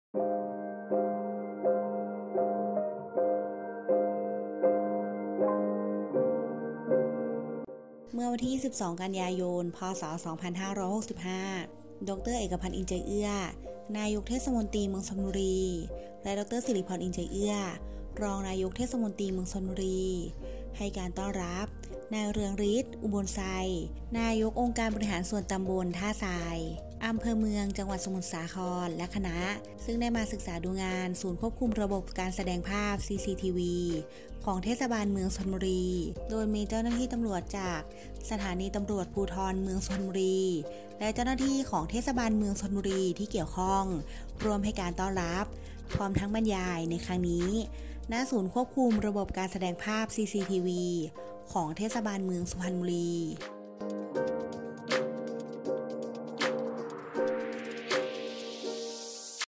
ต้อนรับคณะศึกษาดูงานจาก อบต.ท่าทราย อำเภอเมืองสมุทรสาคร จังหวัดสมุทรสาคร (22ก.ย.65)
#ต้อนรับคณะศึกษาดูงานจาก อบต.ท่าทราย อำเภอเมืองสมุทรสาคร จังหวัดสมุทรสาคร.
            เมื่อวันที่ 22 กันยายน 2565 ดร.เอกพันธุ์ อินทร์ใจเอื้อ นายกเทศมนตรีเมืองสุพรรณบุรี  และ ดร.ศิริพร อินทร์ใจเอื้อ รองนายกเทศมนตรีเมืองสุพรรณบุรี ให้การต้อนรับ นายเรืองฤทธิ์  อุบลไทร นายก อบต.ท่าทราย
อ.เมือง จ.สมุทรสาคร และคณะฯ ซึ่งได้มาศึกษาดูงานศูนย์ควบคุมระบบการแสดงภาพ cctv
ของ ทม.สพ.โดยมีเจ้าหน้าที่ตำรวจ จากสภ.เมืองสุพรรณบุรีและเจ้าหน้าที่ทม.สพ.ที่เกี่ยวข้องร่วมให้การต้อนรับและบรรยายในครั้งนี้ฯ ณ ศูนย์ควบคุมระบบการแสดงภาพ CCTV ของเทศบาลเมืองสุพรรณบุรี